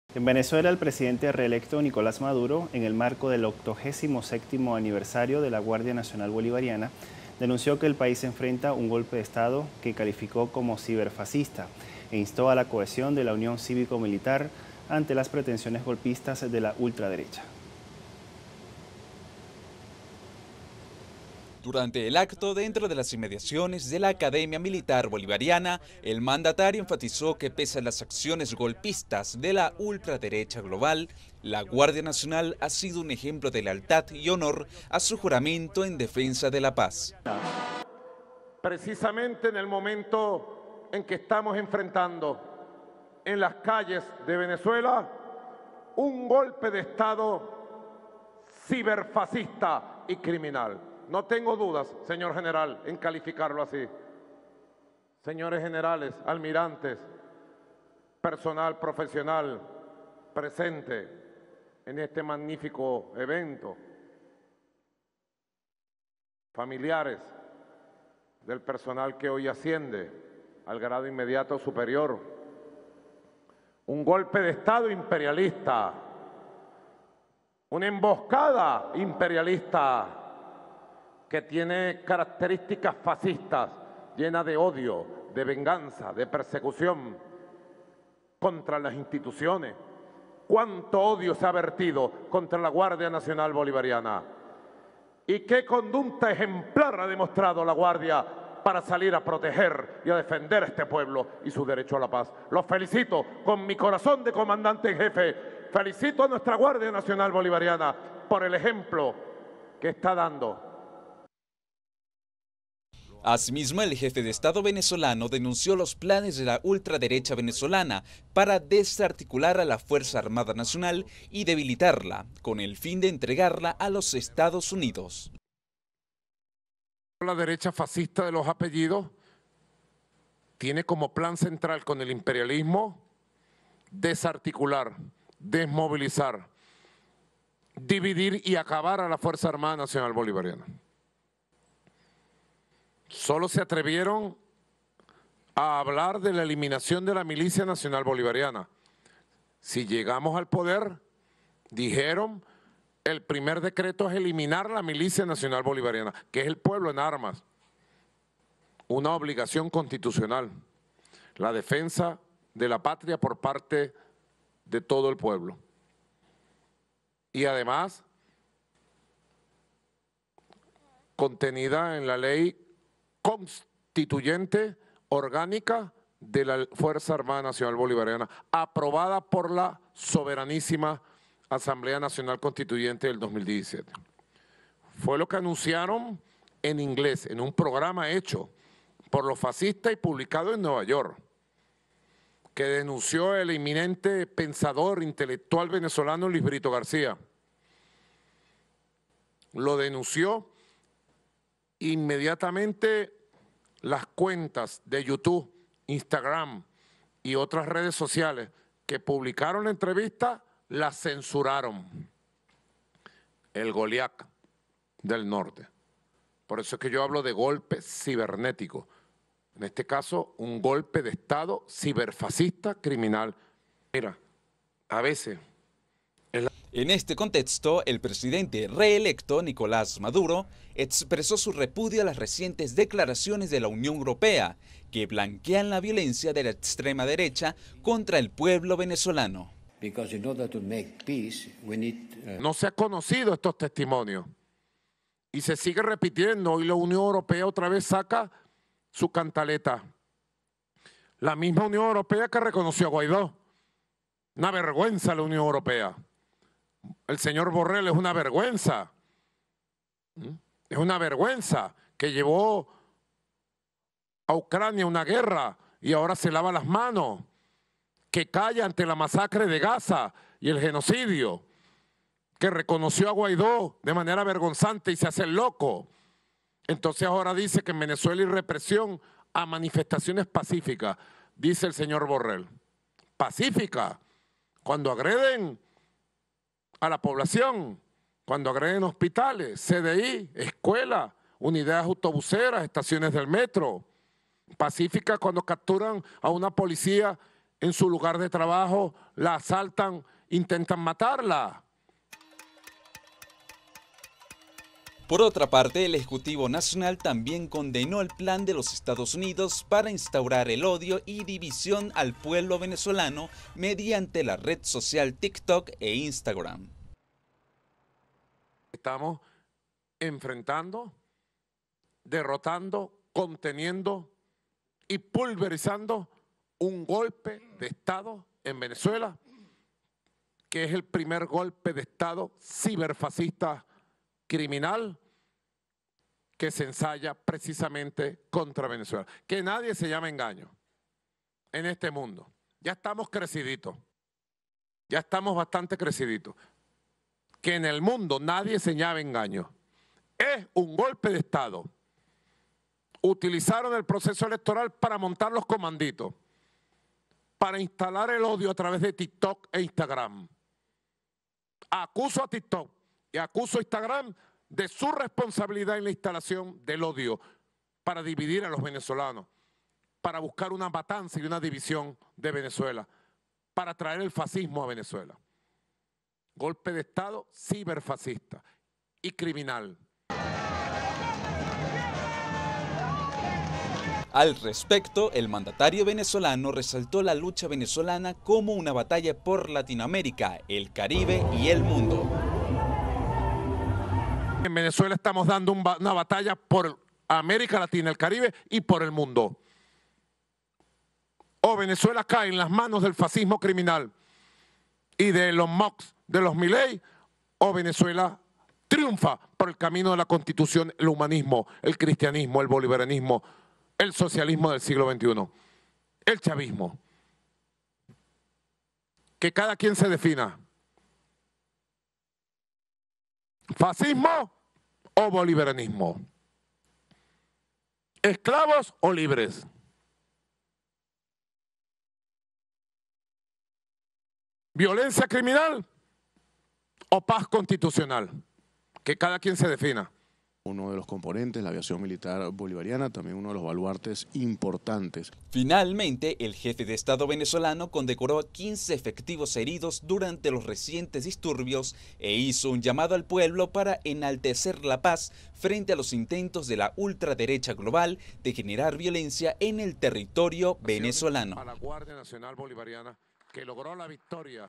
Gobierno venezolano denuncia amenaza de golpe de estado ciberfascista
En el marco del 87 aniversario de la GNB el presidente reelecto Nicolás Maduro llamó a la unión Cívico-Militar ante amenazas de un golpe de estado ciberfascista por parte de la ultraderecha.